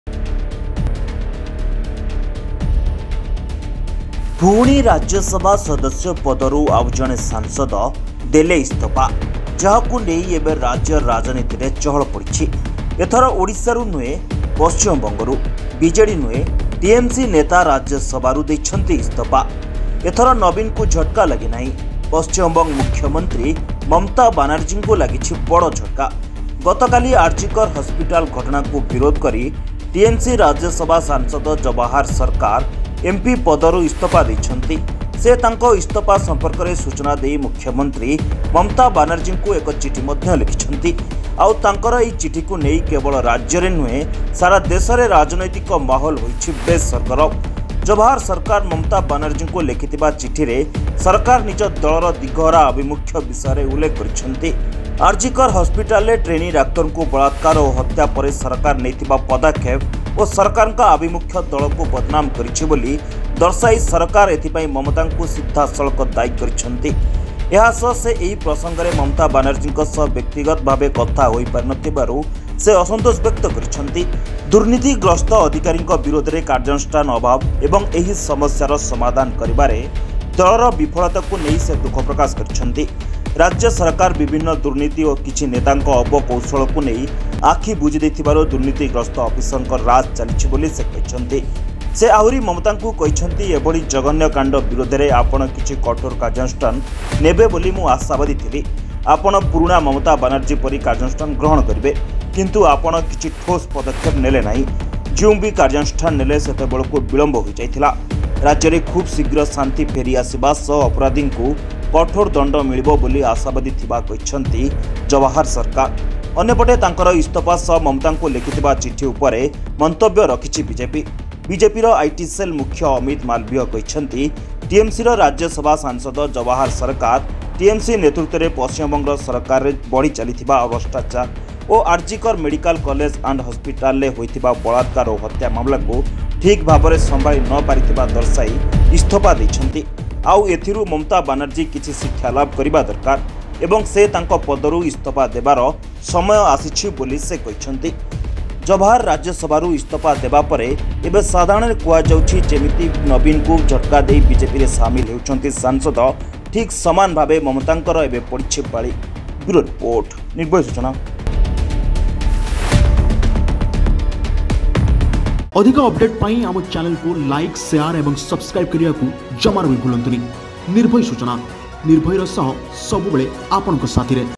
ନବୀନଙ୍କ ପରେ ମମତାଙ୍କ ପାଳି ! ରାଜ୍ୟସଭାରୁ ଇସ୍ତଫା ଦେଲେ ଆଉ ଜଣେ ସାଂସଦ | Nirbhay Suchana
ପୁଣି ରାଜ୍ୟ ସଭା ସଦସ୍ୟ ପଦରୁ ଆଉ ଜଣେ ସାଂସଦ ଦେଲେ ଇସ୍ତଫା । ଯାହାକୁ ନେଇ ଏବେ ରାଜ୍ୟ ରାଜନୀତିରେ ଚହଳ ପଡିଛି। ଏଥର ଓଡ଼ିଶାରୁ ନୁହେଁ , ପଶ୍ଚିମବଙ୍ଗରୁ । ବିଜେଡି ନୁହେଁ ଟିଏମସି ନେତା ରାଜ୍ୟସଭାରୁ ଦେଇଛନ୍ତି ଇସ୍ତଫା । ଏଥର ନବୀନଙ୍କୁ ଝଟକା ଲାଗି ନାହିଁ , ପଶ୍ଚିମବଙ୍ଗ ମୁଖ୍ୟମନ୍ତ୍ରୀ ମମତା ବାନାର୍ଜିଙ୍କୁ ଲାଗିଛି ବଡ଼ ଝଟକା । ଗତକାଲି ଆରଜି କର ହସ୍ପିଟାଲ ଘଟଣାକୁ ବିରୋଧ କରି ଟିଏମସି ରାଜ୍ୟ ସଭା ଏମପି ଜବାହର ସରକାର ଏମପି ପଦରୁ ଇସ୍ତଫା ଦେଇଛନ୍ତି।